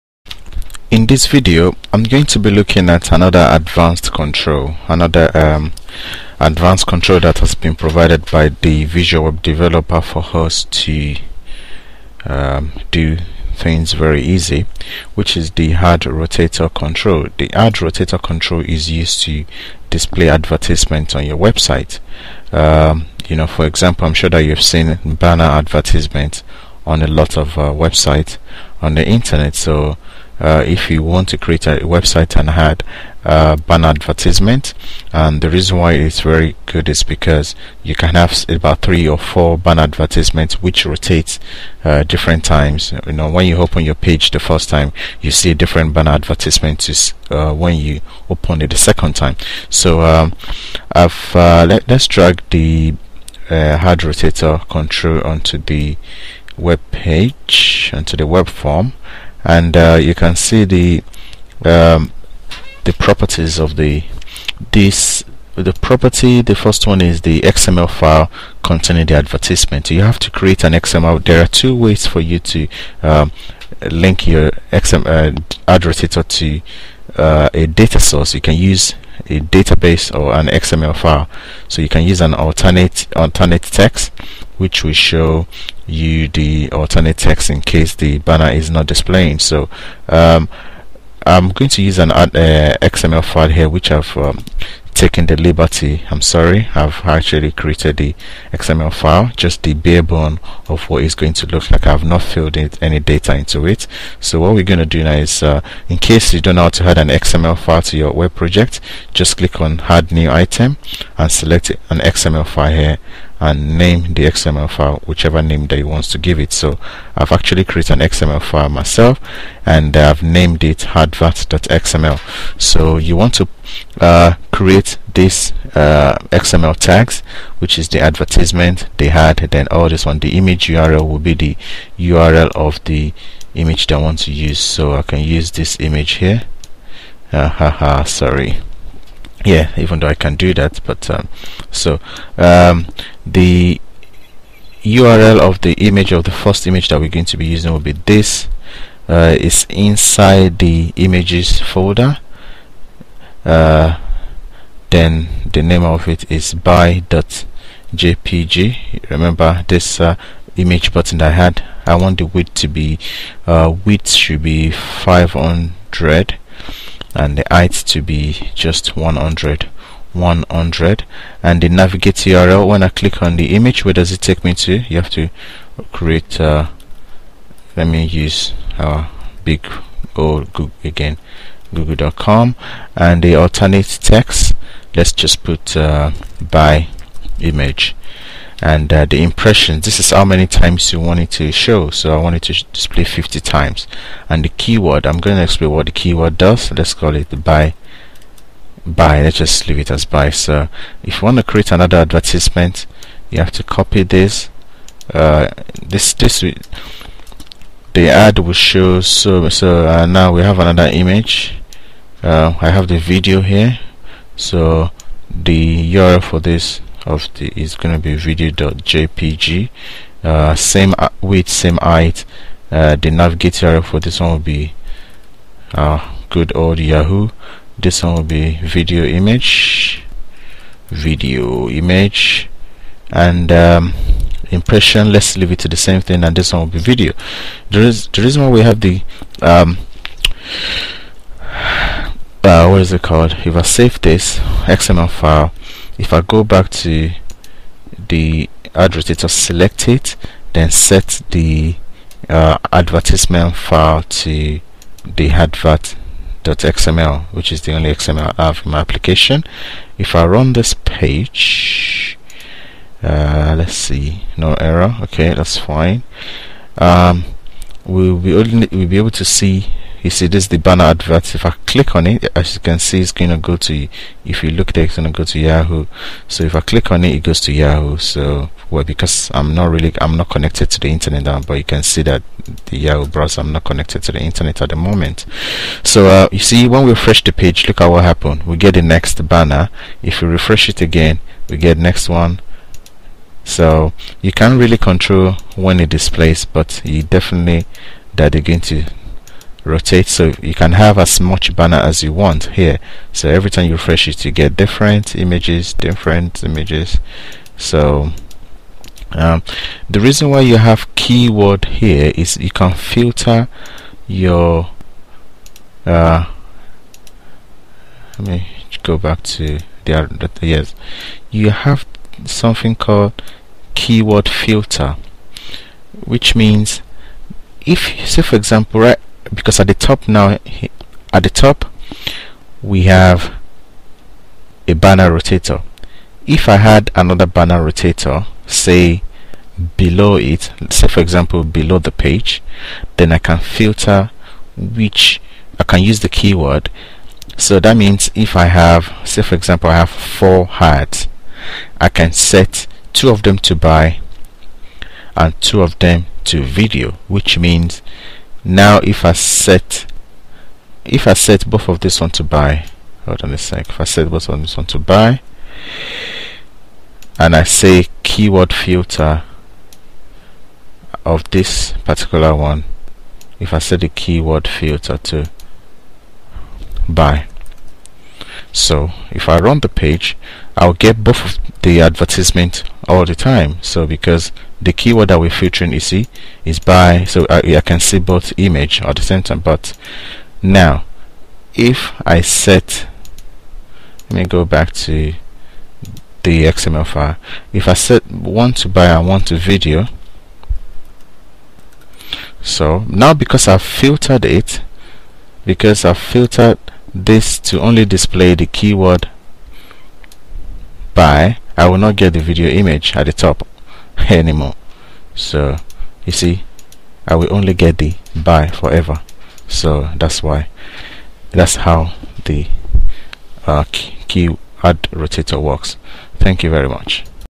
58 - ASP.NET AdRotator Control
how to use the asp.net adrotator control